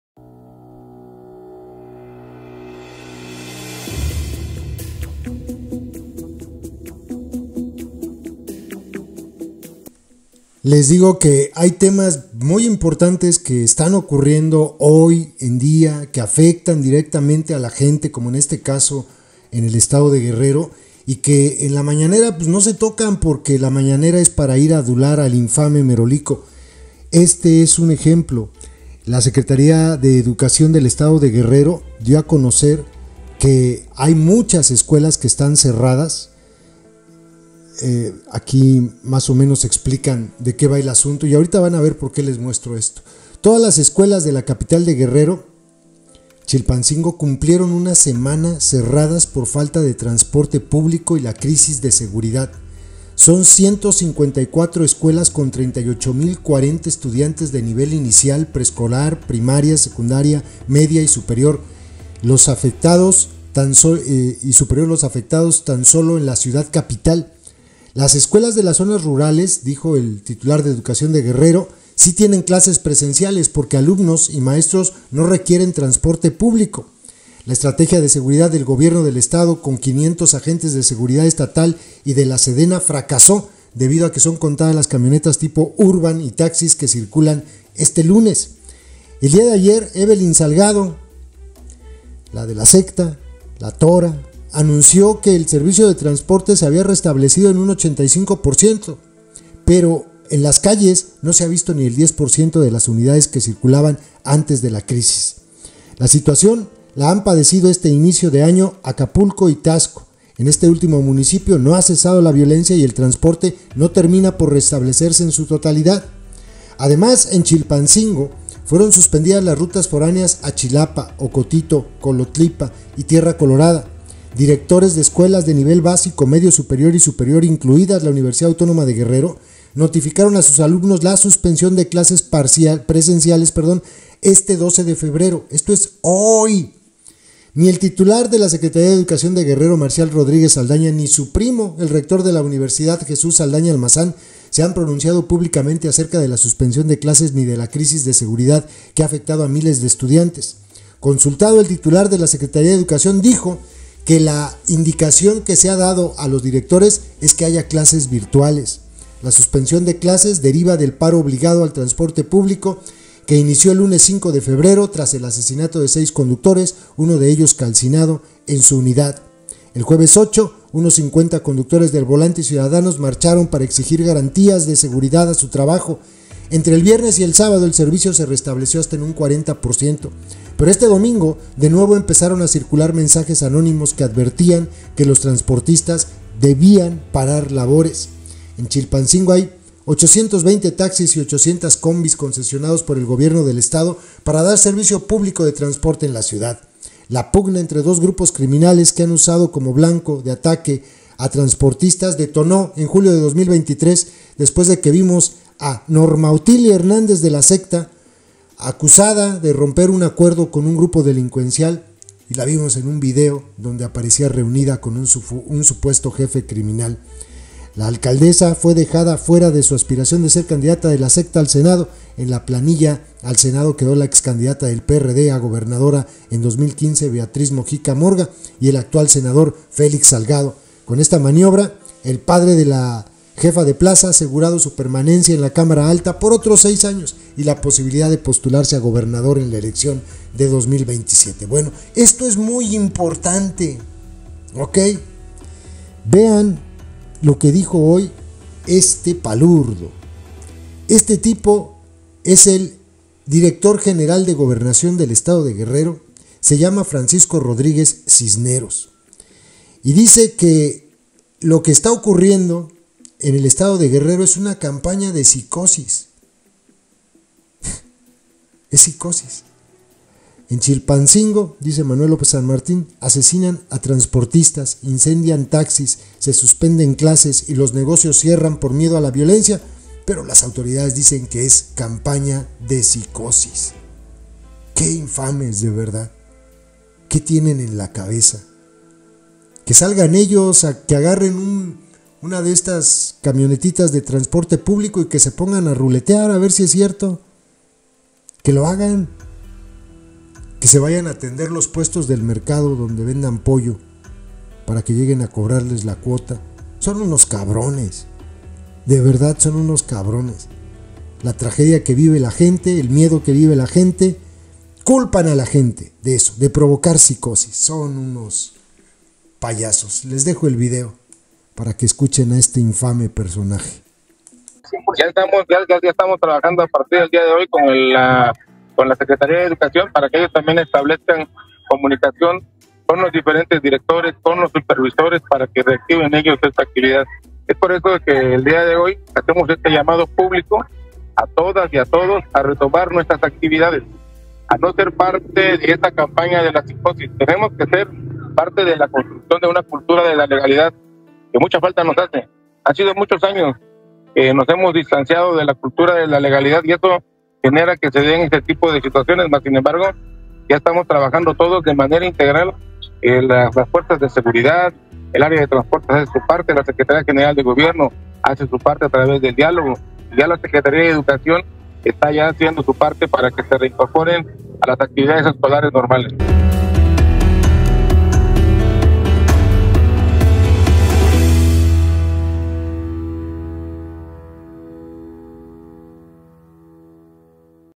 Gobierno de Guerrero dice que es una 'campaña de psicosis' lo que ocurre en Chilpancingo
Para el director general de Gobernación de Guerrero, Francisco Rodríguez Cisneros, todo lo que hemos visto los últimos días en Chilpancingo y en otros municipios del estado, es una "campaña de psicosis". Dice que, según él, ya hay condiciones para que regresen a clases los alumnos y se restablezca el transporte público. ¿En qué mundo vive?